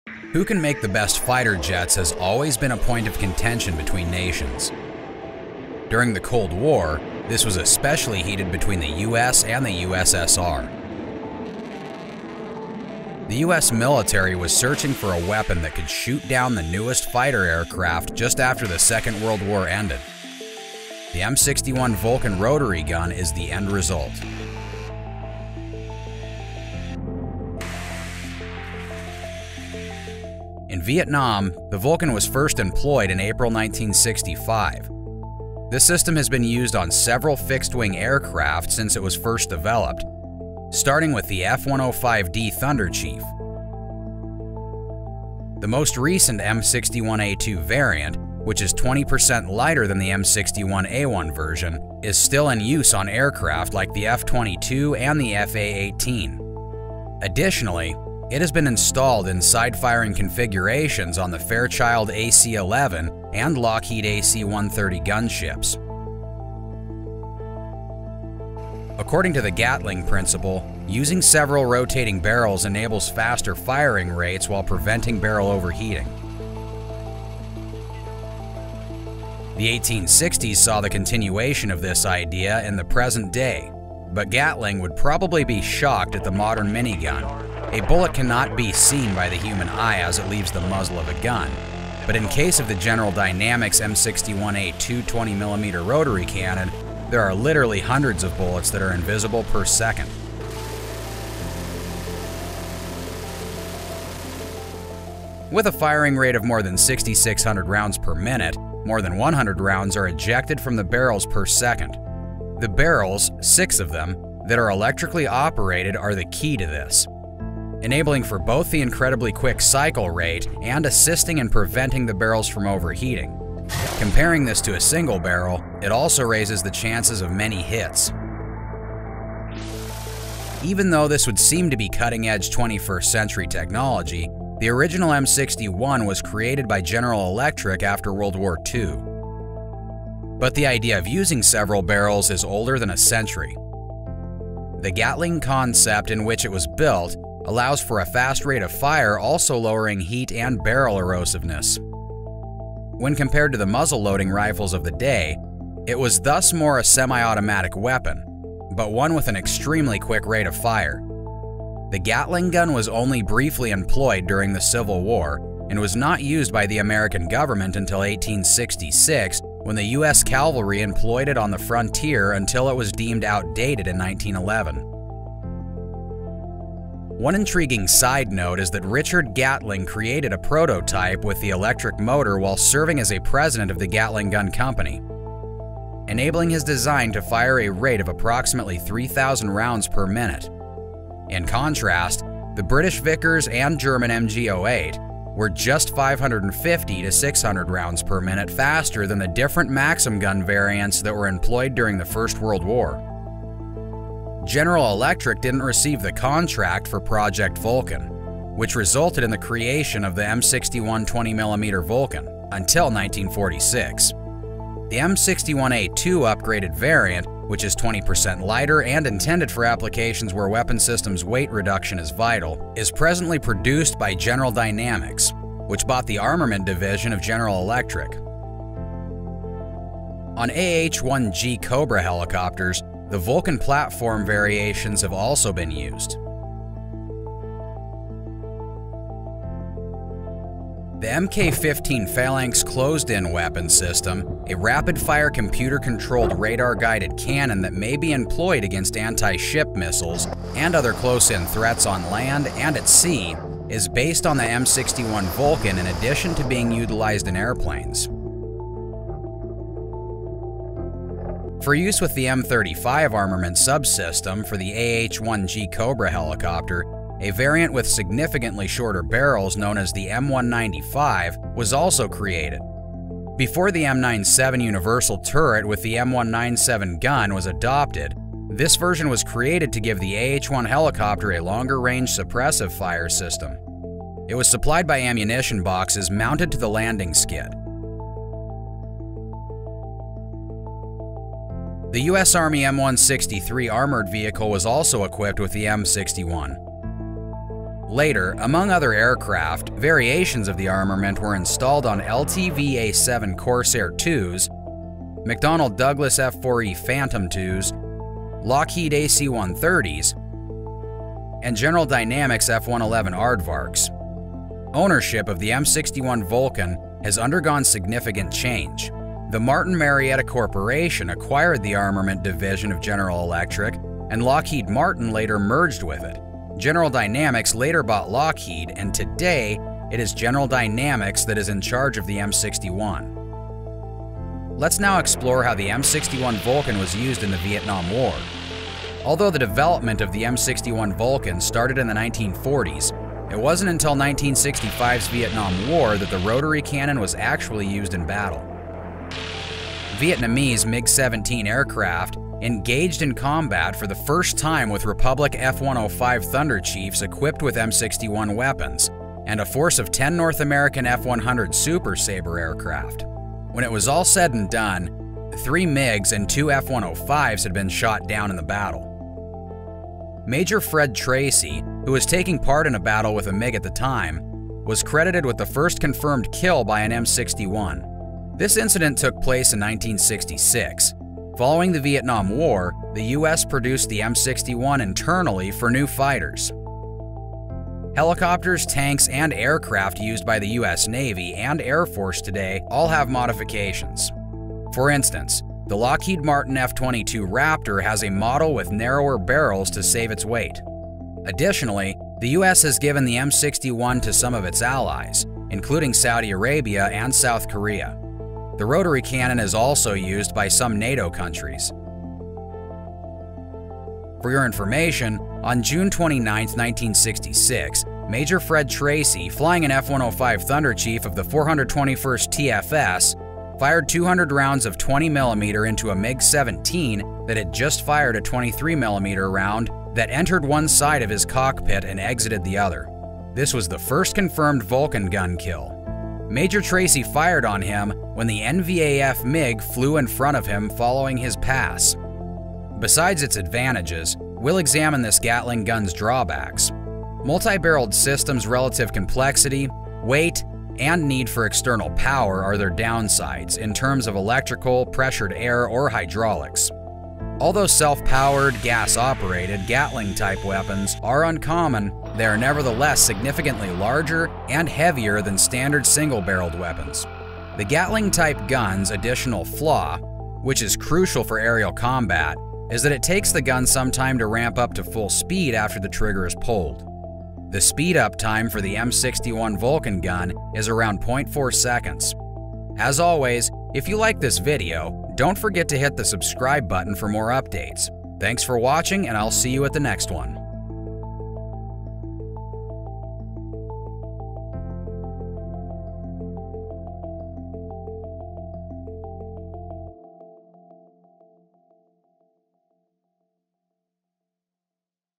M61 Vulcan: The Gun That Can Fire 6,000 Round Per Minute
The M61 Vulcan is a rotary cannon that uses 20 x 102 mm cartridges and is powered by hydraulics. It was originally designed to use linked ammunition, but later versions use a linkless feeding system. With a range of around 600 meters, the weapon fires rounds as the barrels rotate, allowing for a high rate of fire. It can shoot up to 100 rounds per second, or 6,000 rounds per minute, and is known for its reliability with a jam or failure rate of only one in every 10,000 rounds. To manage the high ammo consumption, a burst controller was added by General Electric to allow pilots to control the amount of rounds used. The gun also has been used as a defensive armament on military aircraft and has been installed in a variety of aircraft including the F-104 Starfighter, F-4 Phantom II, F-15 Eagle, F-16 Fighting Falcon, and F/A-18 Hornet. The M61 may seem like a modern invention, but it was actually first developed by General Electric in the post-World War II era, and first deployed in 1956. However, the idea of using multiple barrels for a weapon dates back even further, over a century before the M61's creation.